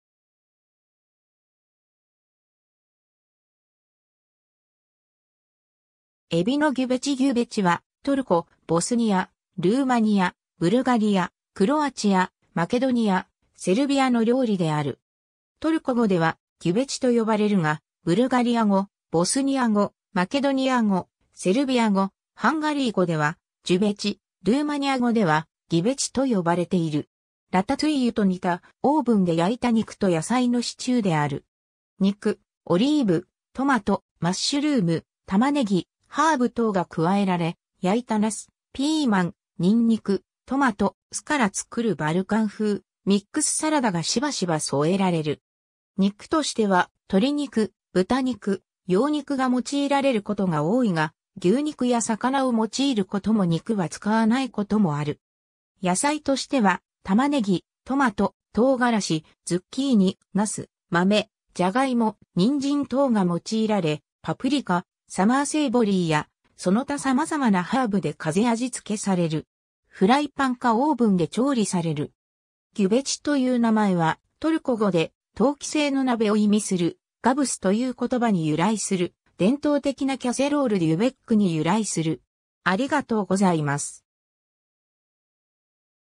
ギュベチ
エビのギュベチ ギュベチ(トルコ語: güveç, [ɡyˈvet͡ʃ];、ブルガリア語: гювеч, [ɟuˈvɛt͡ʃ]、ボスニア語: đuveč, [ɟuˈvɛt͡ʃ]、マケドニア語: ѓувеч, [ˈɟuvɛt͡ʃ]、セルビア語: ђувеч, [ˈd͡ʑuʋɛt͡ʃ]、ルーマニア語: ghiveci, [ɡivet͡ʃ]、ハンガリー語: gyuvecs, [ˈɟuvet͡ʃ])は、トルコ、ボスニア、ルーマニア、ブルガリア、クロアチア、マケドニア、セルビアの料理である。
トルコ語ではギュヴェチと呼ばれるが、ブルガリア語、ボスニア語、マケドニア語、セルビア語、ハンガリー語ではジュヴェチ、ルーマニア語ではギヴェチと呼ばれている。
ラタトゥイユと似た、オーブンで焼いた肉と野菜のシチューである。
肉、オリーブ、トマト、マッシュルーム、タマネギ、ハーブ等が加えられ、焼いたナス、ピーマン、ニンニク、トマト、酢から作るバルカン風ミックスサラダがしばしば添えられる。
 肉としては、鶏肉、豚肉、羊肉が用いられることが多いが、牛肉や魚を用いることも肉は使わないこともある。
野菜としては、タマネギ、トマト、唐辛子、ズッキーニ、ナス、豆、ジャガイモ、ニンジン等が用いられ、パプリカ、サマーセイボリーやその他様々なハーブで風味付けされる。
フライパンかオーブンで調理される。
 ギュベチという名前は、トルコ語で「陶器製の鍋」を意味するgüveçという言葉に由来する伝統的なキャセロールđuvečに由来する。